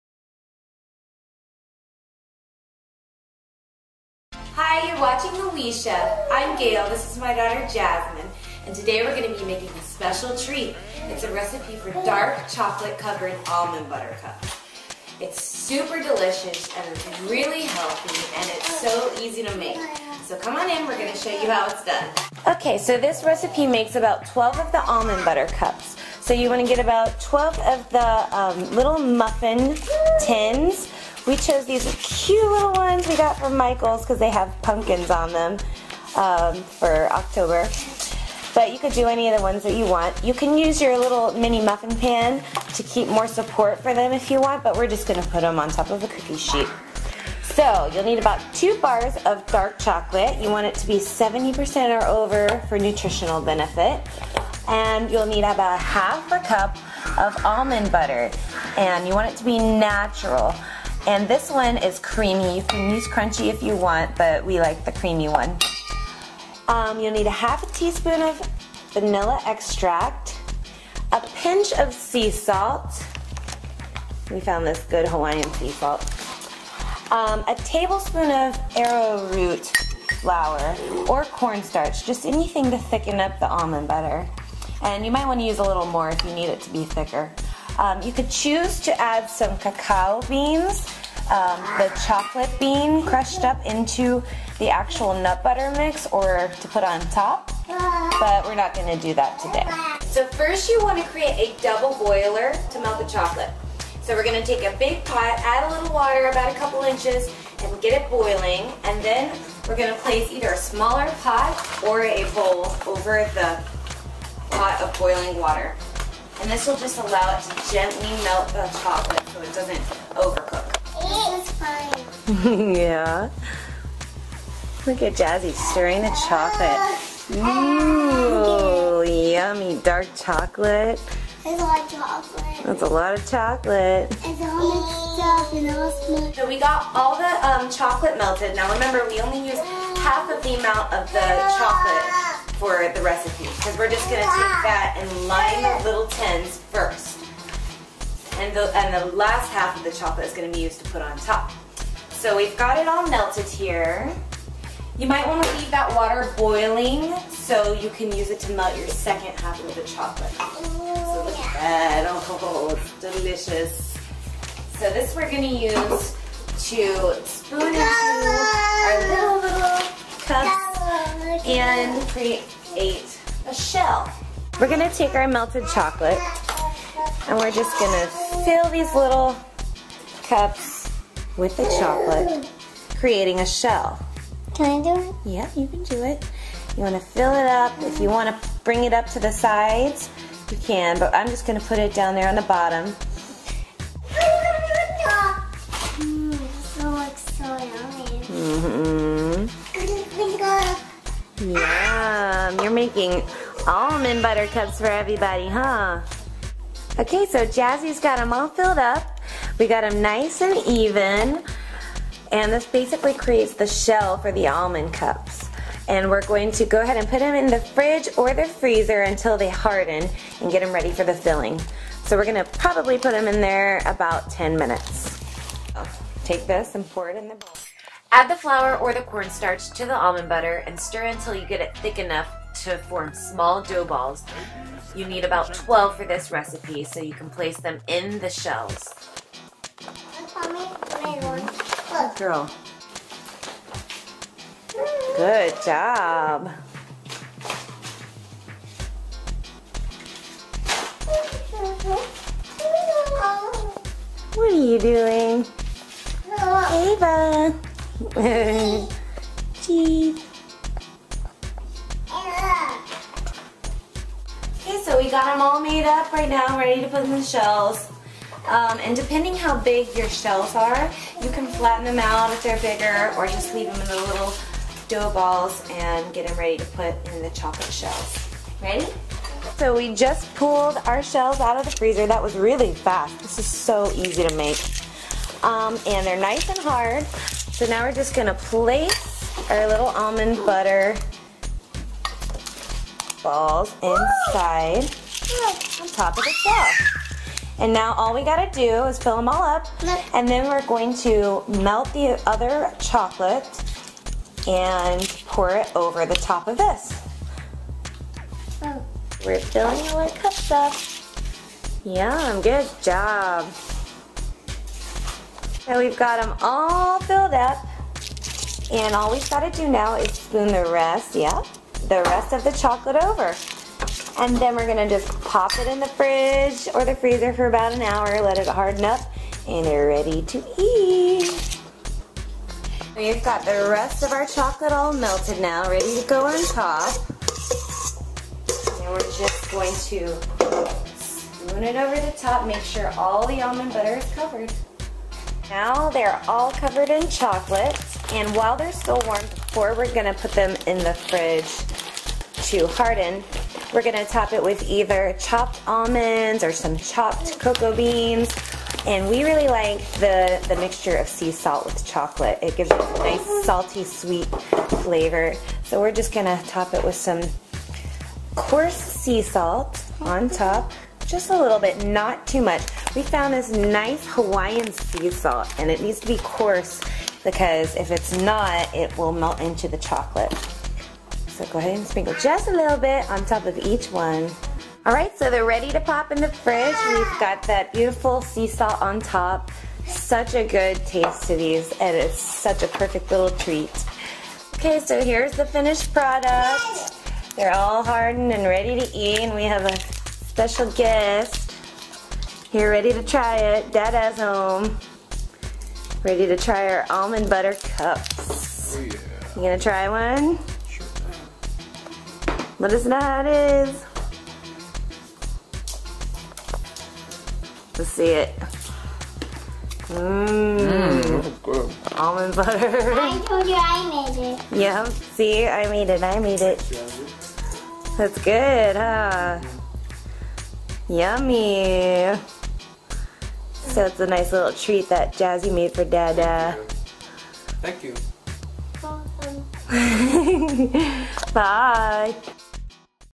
Dark Chocolate Covered Almond Butter Cups - Healthy Dessert Recipes
Watch this fun lady with her pretty little girl take us to chef school and teach us how to make Dark Chocolate Covered Almond Butter Cups...one of my favorite Healthy Dessert Recipes.